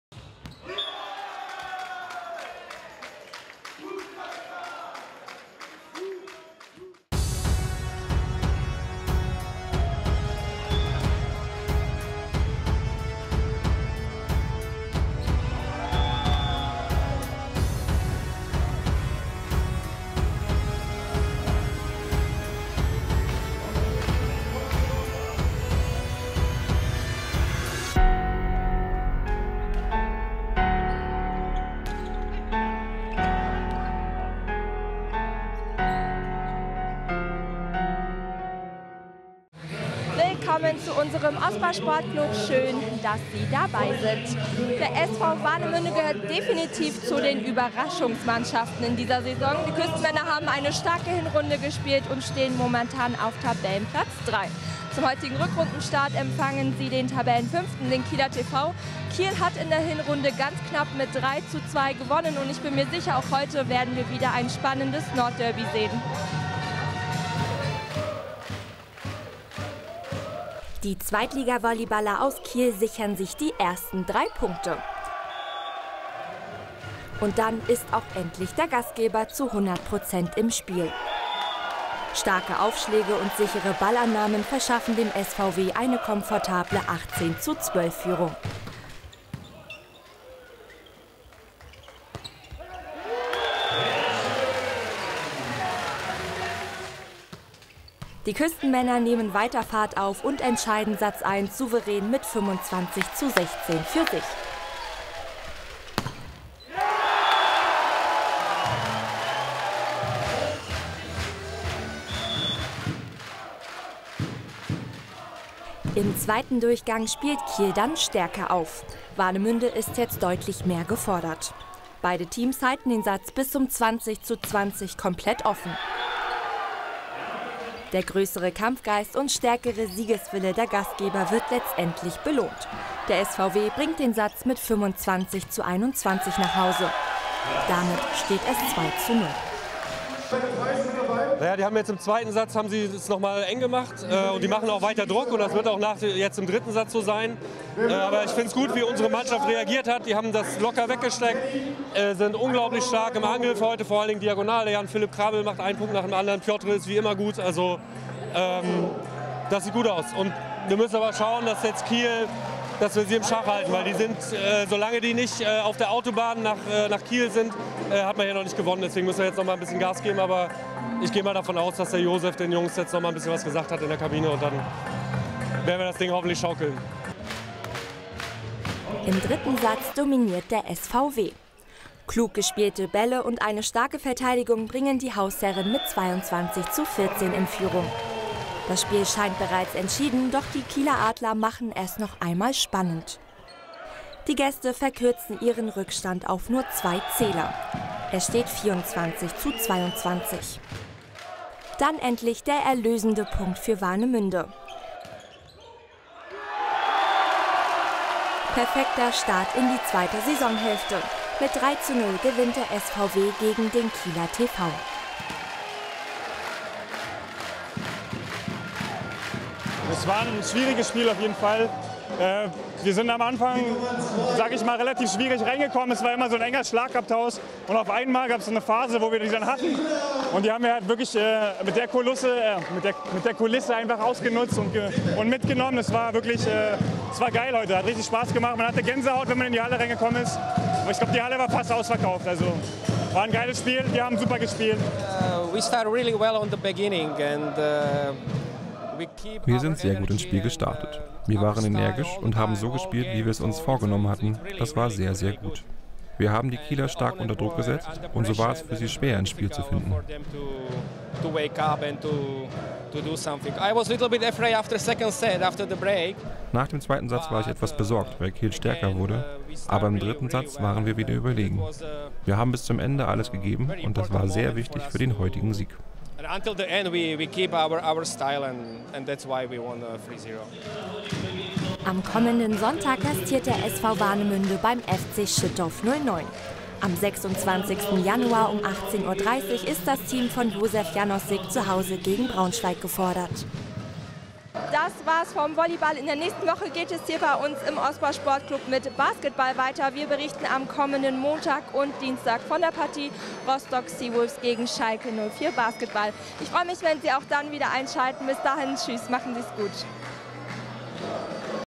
OSPA Sportclub: SV Warnemünde Volleyball - Kieler TV
In der neuen Episode des OSPA Sportclubs schauen wir zurück auf das Spiel des SV Warnemünde - Volleyball gegen den Kieler TV.